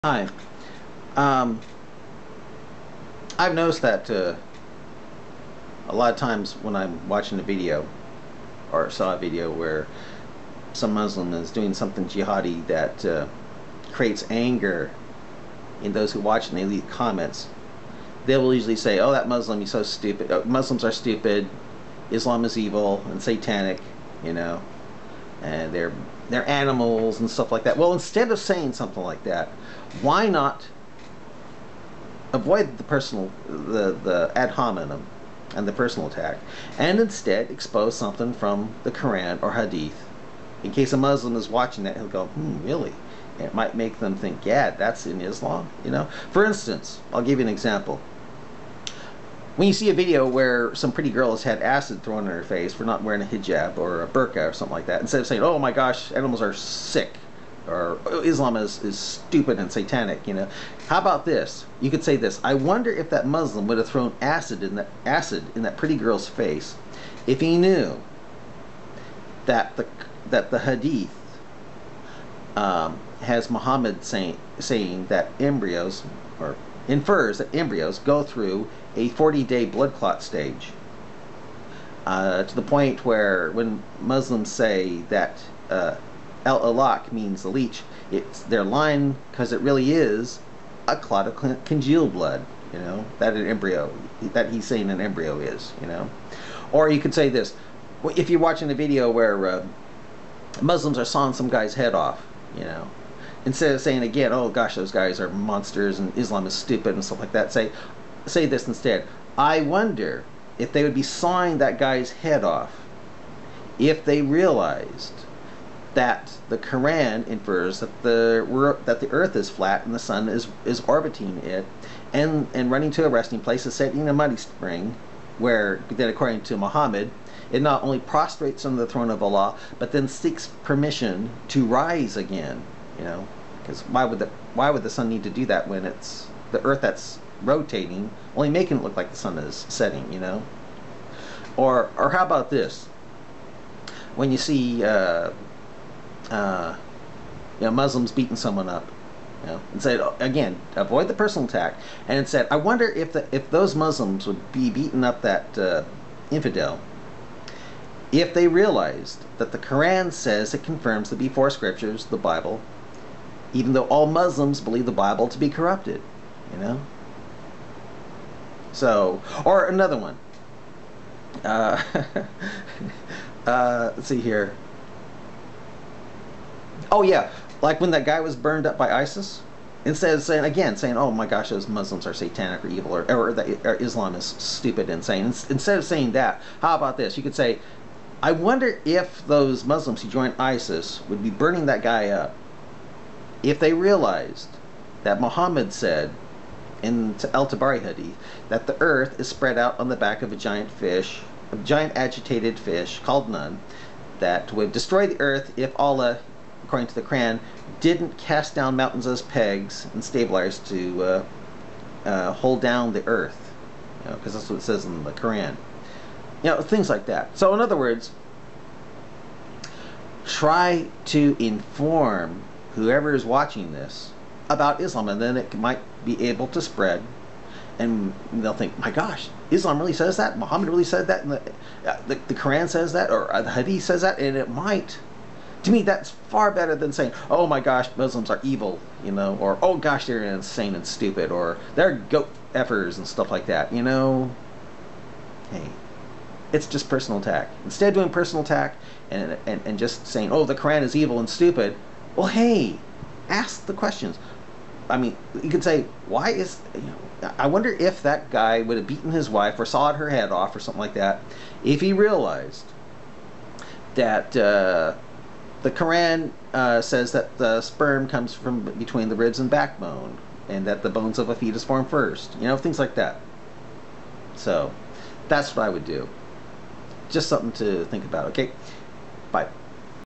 O NON-Muslim! Avoid The Personal Attack On Muslims and Islam, And, Instead...
Instead of commenting and using personal attack on Muslims and Islam when you see a video or article of a Muslim doing something you object to, why not ask a question, like, "I wonder if that Muslim would have sawed his head off if he knew that the Quran infers in Quran 36:40-42 that the sun and moon travel in boats similar to that of Noah?", etc.